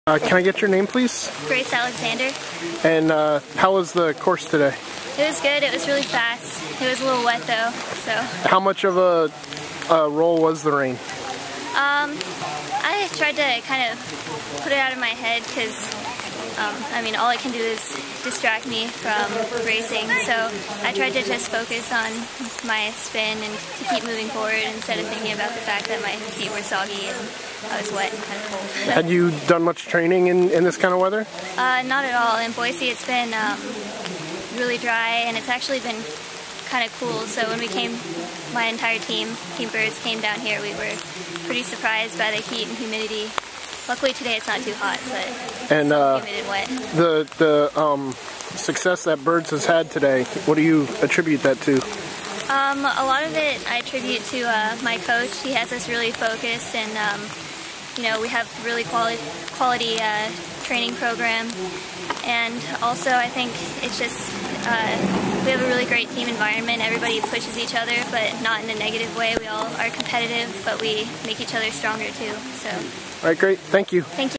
Grace Alexander won the junior women 17-18 time trial.MP4
Grace Alexander talks about winning the junior women 17-18 time trial, the success of the Boise Young Riders Development Squad and the weather at Lake Strom Thurmond and Dam.